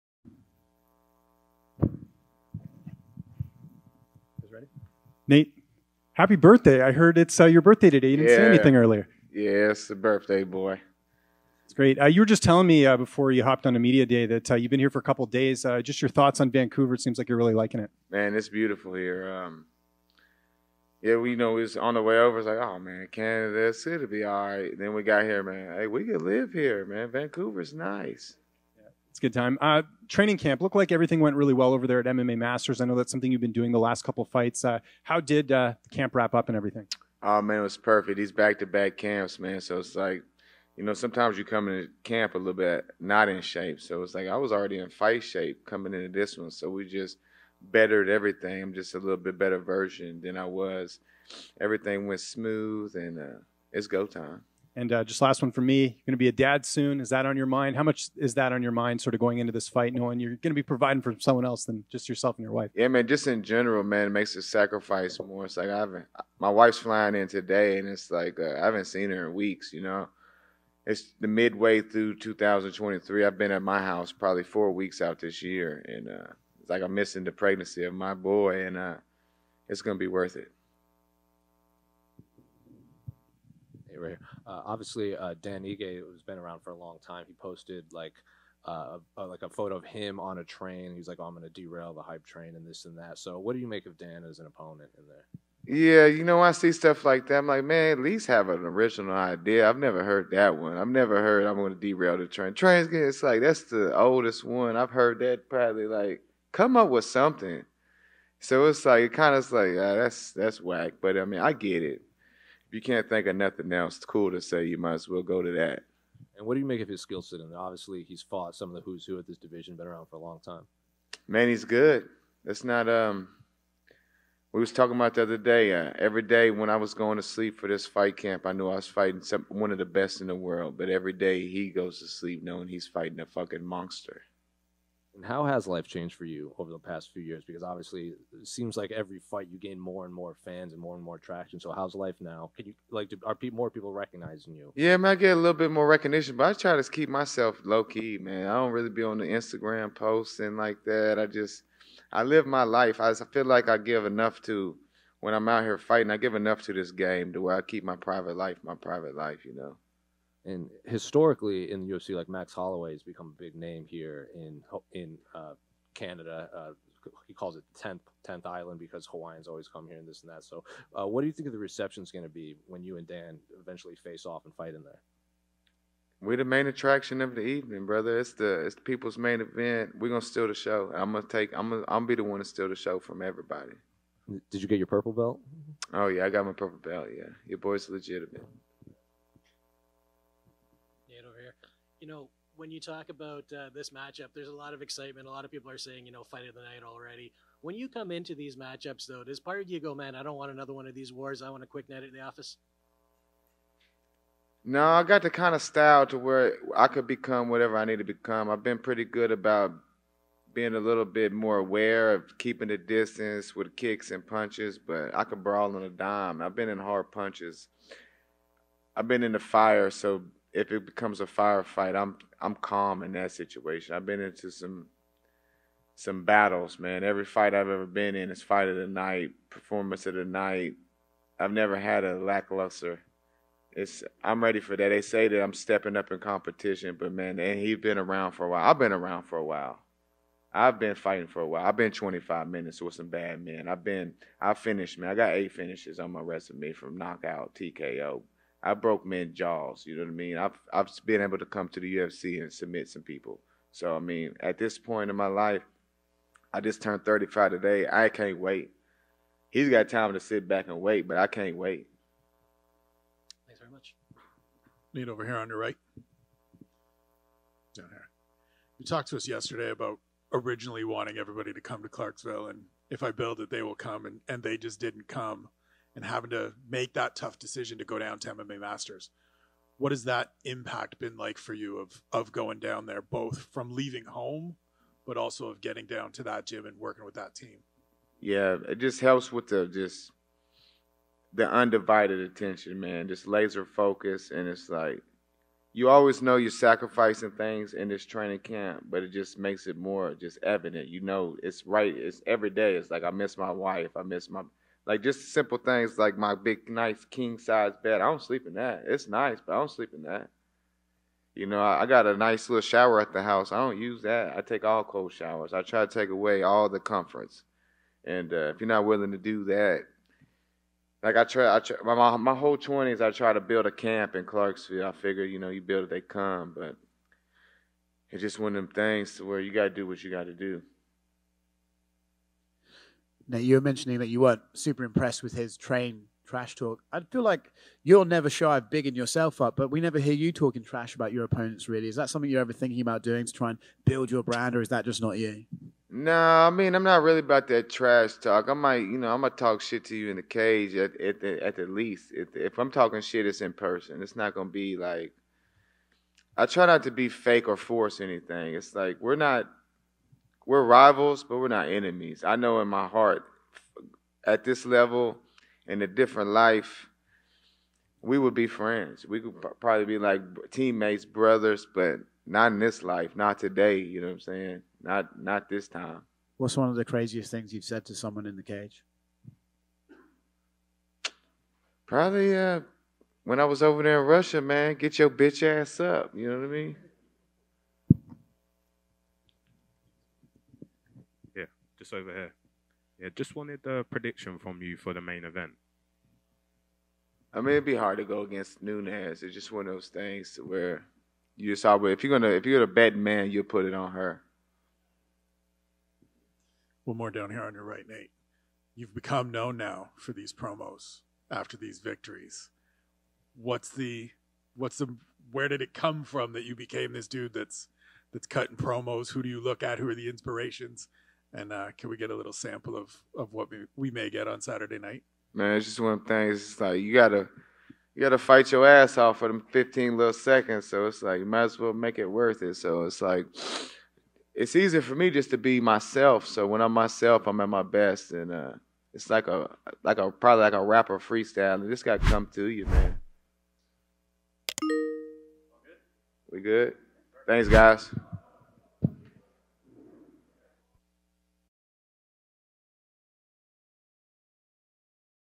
Nate Landwehr | UFC 289 Media Day Interview
Nate Landwehr speaks with the media about his fight against Dan Ige at UFC 289 media day ahead of his fight at UFC 289 on Saturday, June 10 at Rogers Arena, Vancouver.